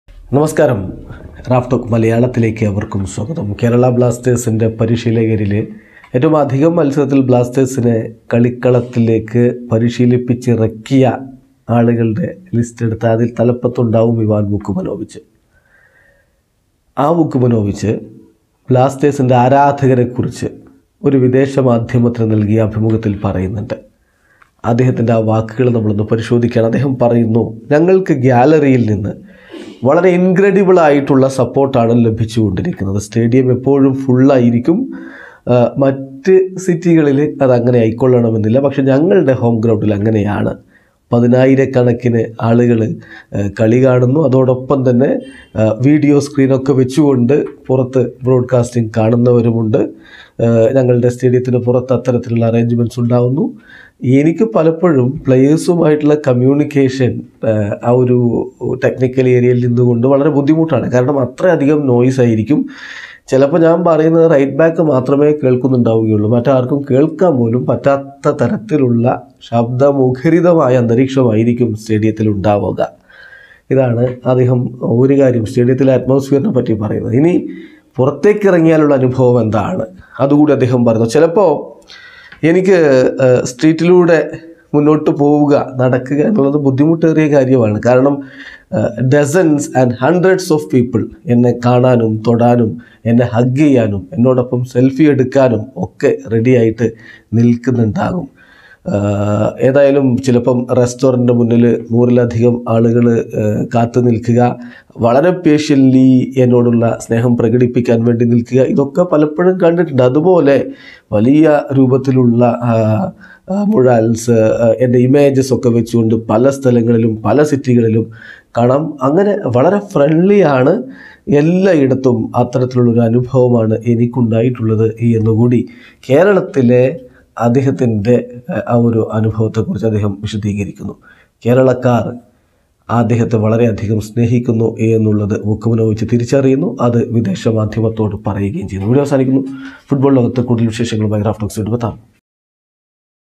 ചിലപ്പോൾ കളിക്കാരുമായി കമ്മ്യൂണിക്കേറ്റ് ചെയ്യാൻ പോലും ആവാറില്ല: ഇവാൻ | Ivan Vukomanovic | KBFC
Kerala blasters
Ivan Vukomanovic
ISL